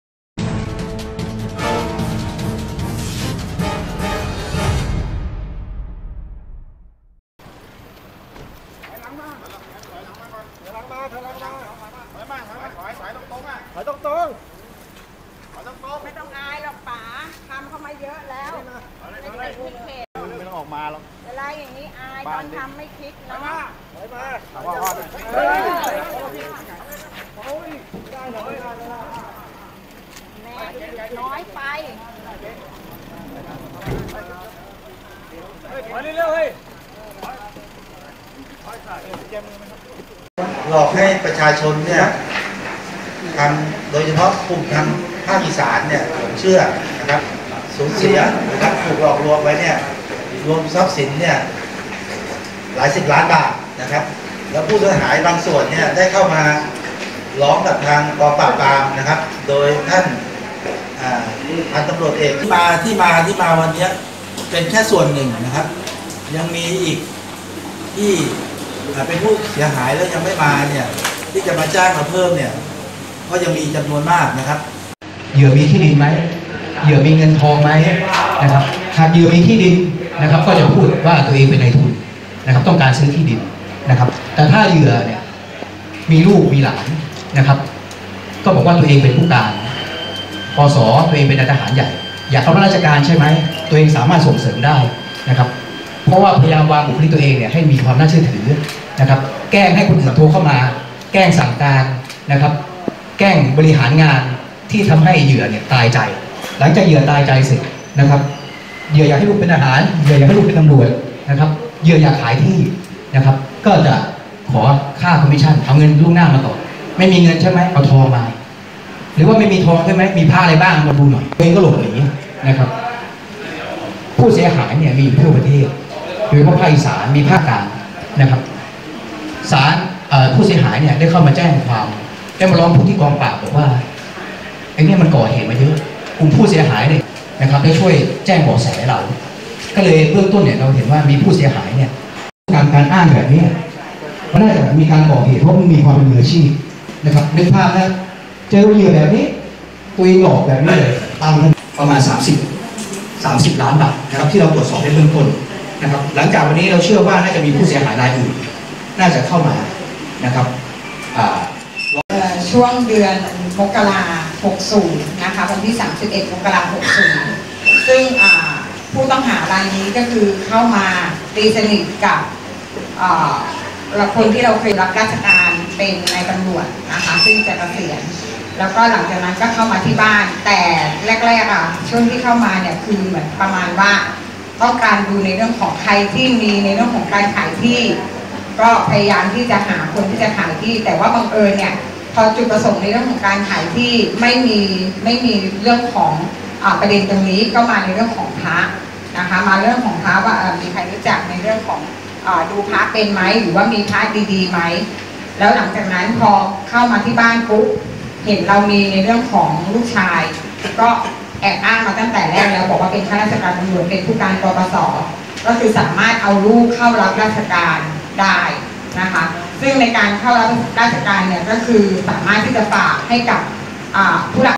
จับผัวเมีย 18 มงกุฎตระเวนหลอกคนทั่วประเทศสูญ 30 ล้าน เจ้าทุกข์แค้นโดดชกหน้าหงาย
14/11/2018 จับผัวเมีย 18 มงกุฎตระเวนหลอกคนทั่วประเทศสูญ 30 ล้าน เจ้าทุกข์แค้นโดดชกหน้าหงาย
MGR online - ตำรวจกองปราบปราม ตามจับผัวเมีย 18 มงกุฎ อ้างเป็นผู้การ ปส. รู้จักคนใหญ่คนโต หลอกเอาทรัพย์เหยื่อสารพัดวิธี เสียหายกว่า 30 ล้าน พบมีหมายจับติดตัว 8 คดี เจ้าตัวยังอ้างเฉยผู้เสียหายมอบเงินทองและทรัพย์สินให้ด้วยความเสน่หาเอง เจ้าทุกข์สุดทนปรี่เข้าชกหน้าหงาย